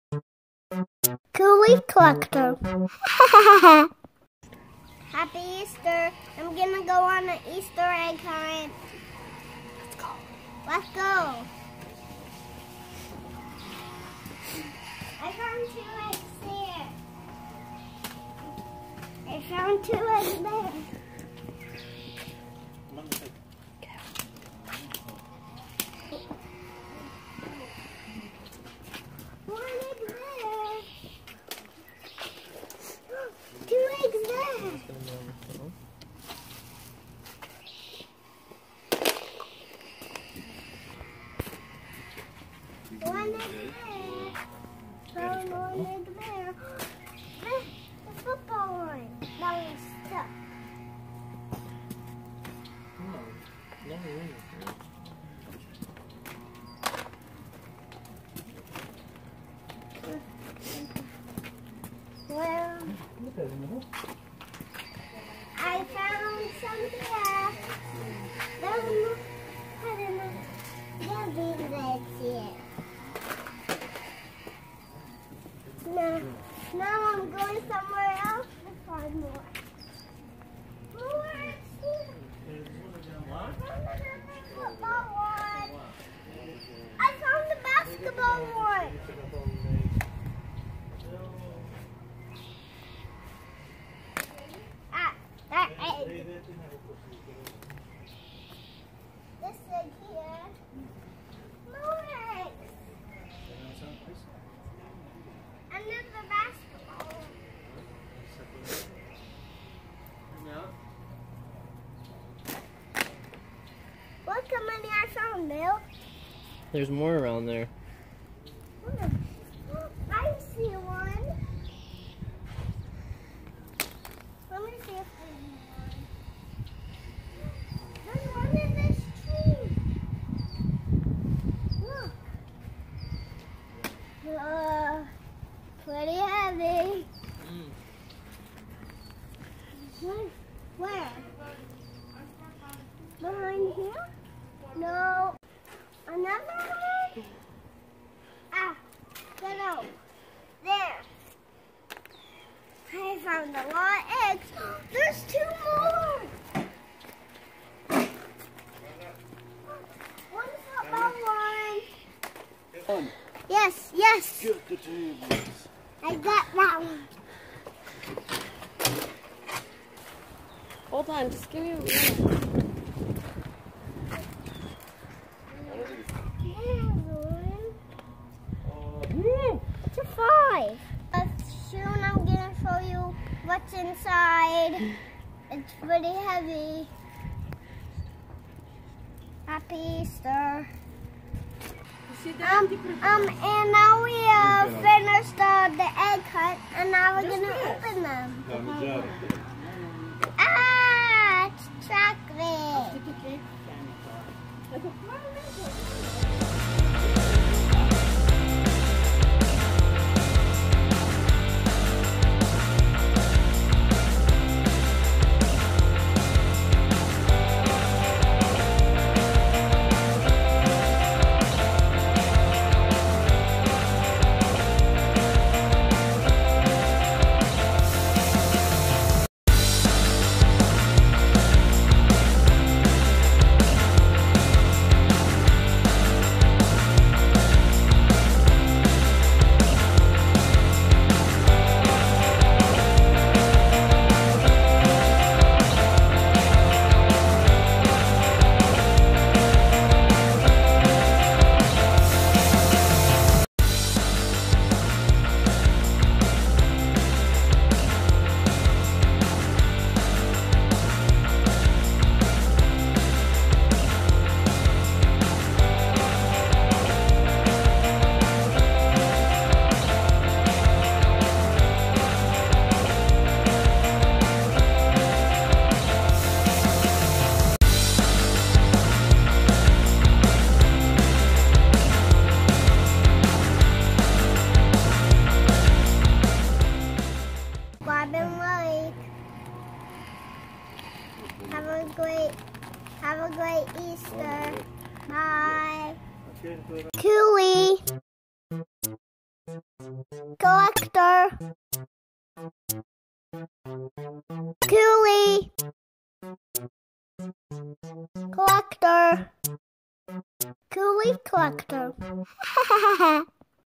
Kouli's Easter EGGtravaganza
Easter Egg Hunt with Kouli Kollector! Join Kouli on his Adventure through the backyard to find all the eggs. Happy Easter Everyone!